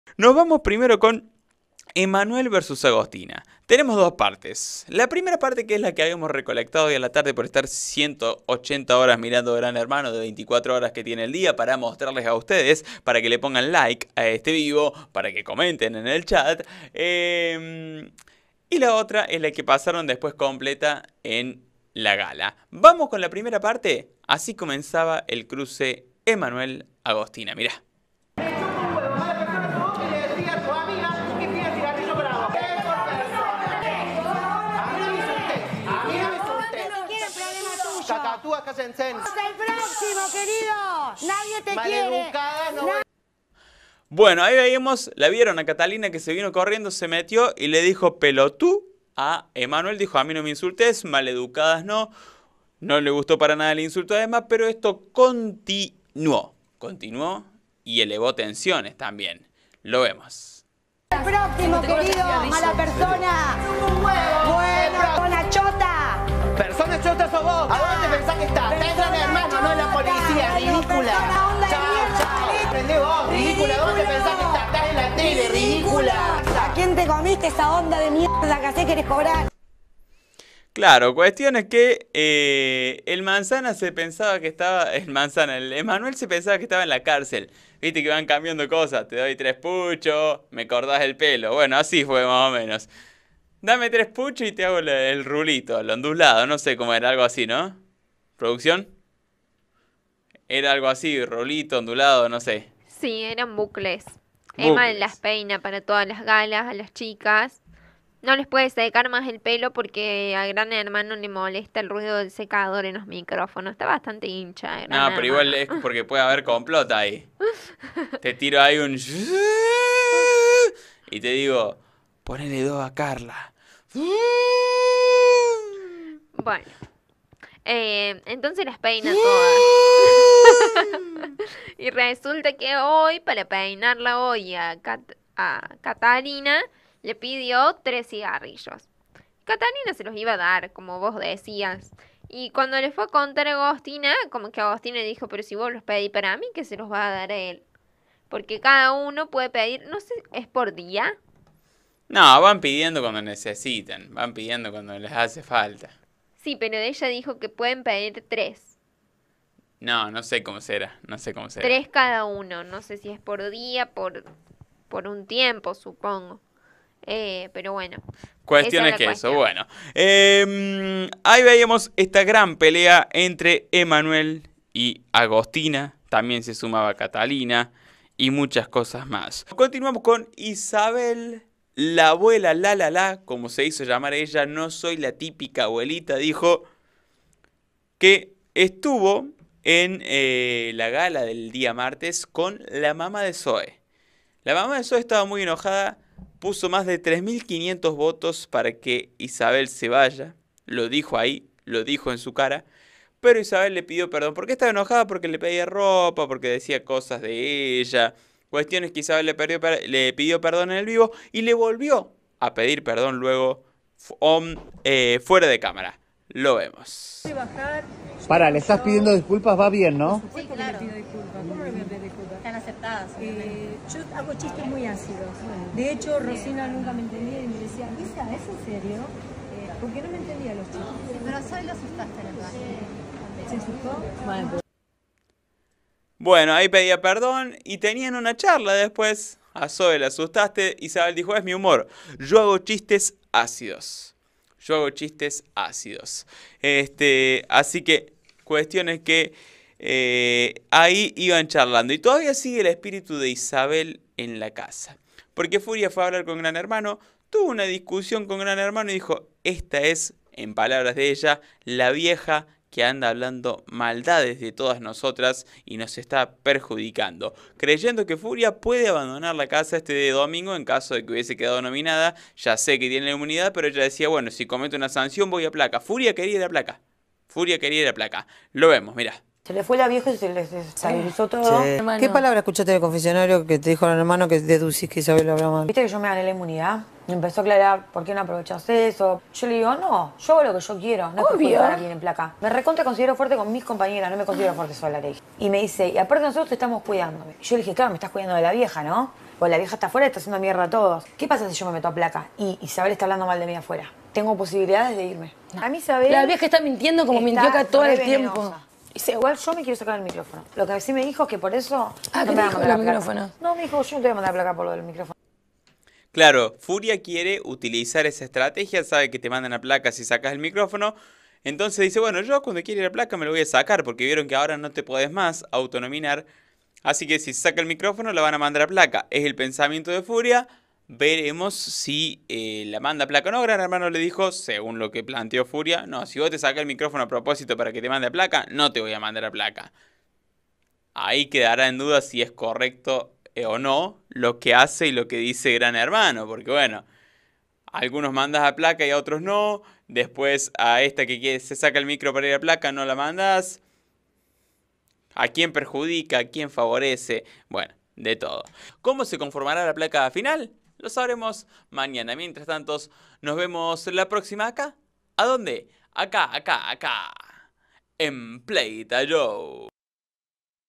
EMMA y AGOSTINA se dijeron DE TODO | GRAN HERMANO 2023
Te contamos TODO lo que se DIJERON EMMA y AGOSTINA. Los hermanitos tuvieron una FUERTE discusión y no dudaron en decirse DE TODO. ¿QUÉ PASÓ?

Todo esto y más te lo contamos en el video, MIRALO HASTA EL FINAL y SORPRENDETE 😱

¡También te invitamos a contarnos qué video sobre qué participante te gustaría que hagamos!